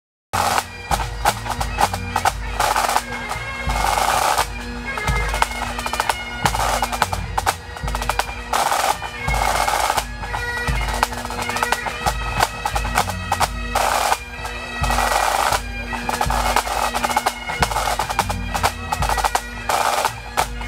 WPB 2019 - Free Practice 4K/UHD]
This is the drum section of an unknown band limbering up